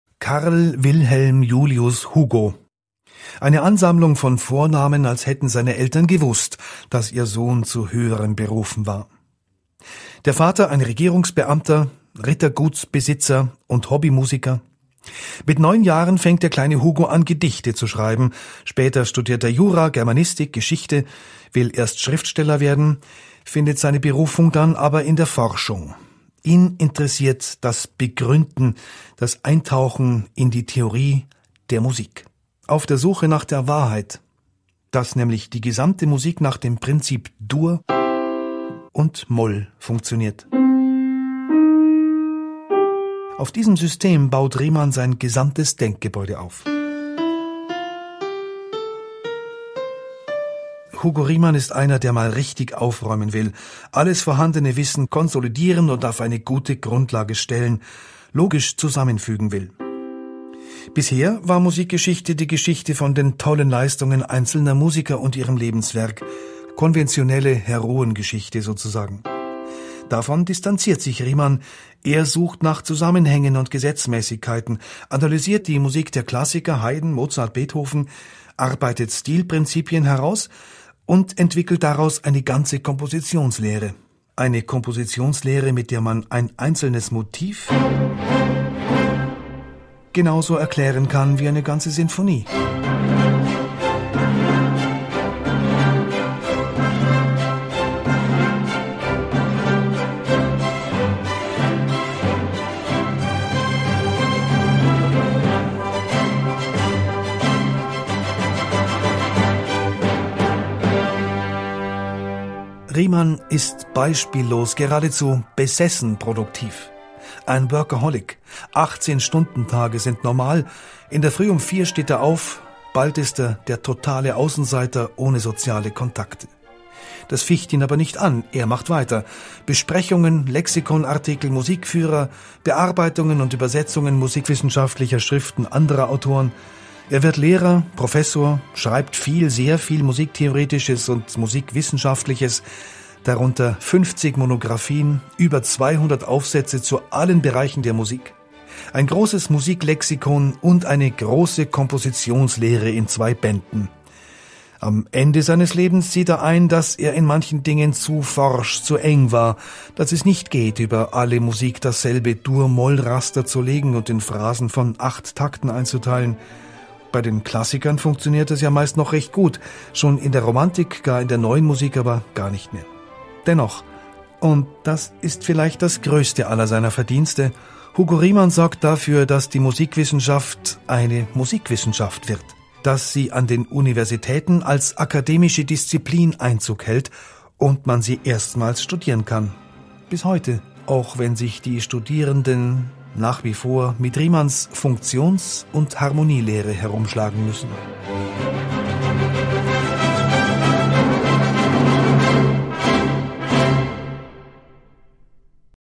18.07.1849 Todestag von Hugo Riemann. Hugo Riemann begründete die moderne Musikwissenschaft.
Sein Musiklexikon ist bekannt und berüchtigt – und ein Klassiker. Riemann starb vor genau 100 Jahren. Eine Annäherung.

Autor: Florian Hauser